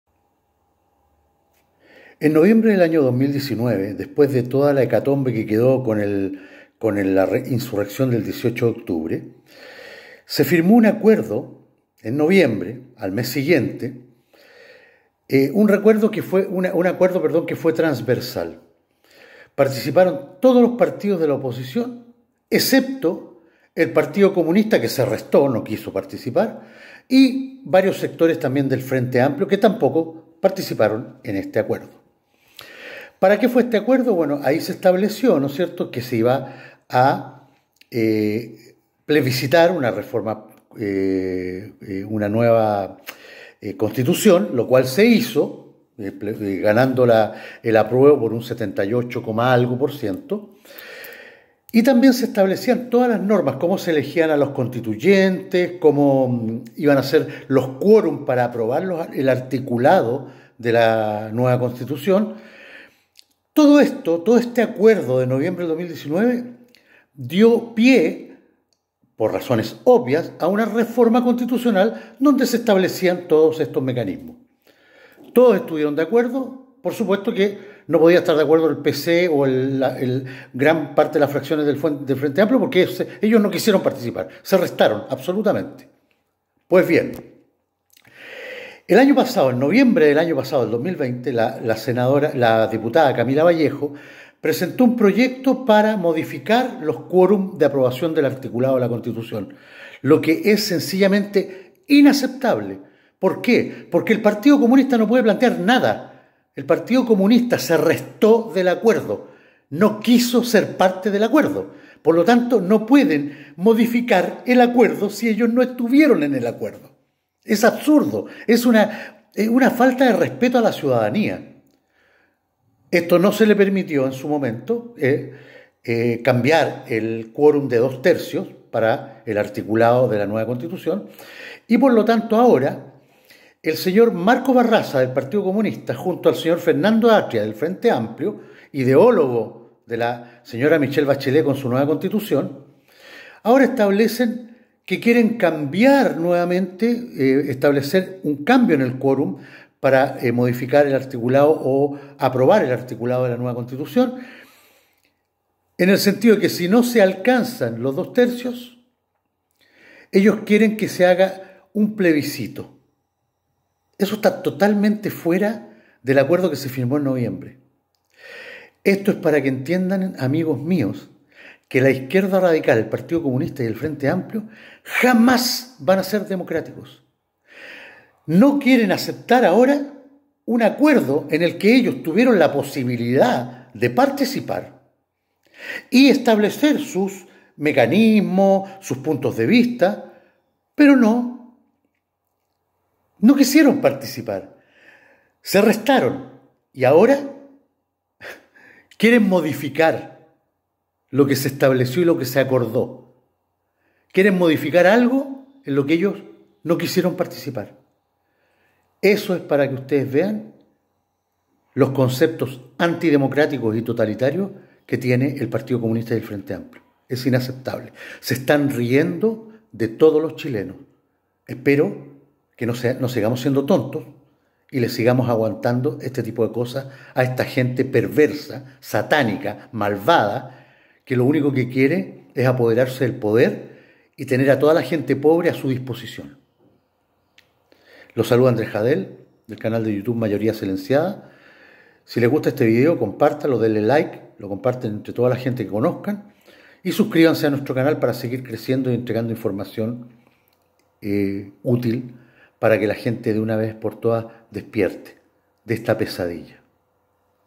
QUORUM DE 2/3 NUEVA CONSTITUCION - IZQUIERDA NUEVAMENTE QUIERE HACER TRAMPA
Después de haberse restado del acuerdo transversal de partidos políticos firmado el 19 de Noviembre de 2019, en donde surgió la reforma constitucional para llamar a plebiscito y fijar las normas de redacción y aprobación del articulado de la nueva constitución, hoy el Partido Comunista y el ala dura del Frente Amplio pretenden modificar el quorum de 2/3 establecidos, planteando que al no lograrse los 2/3, los articulados en conflicto se deberían plebiscitar. Una nueva jugada sucia de la izquierda radical de nuestro país.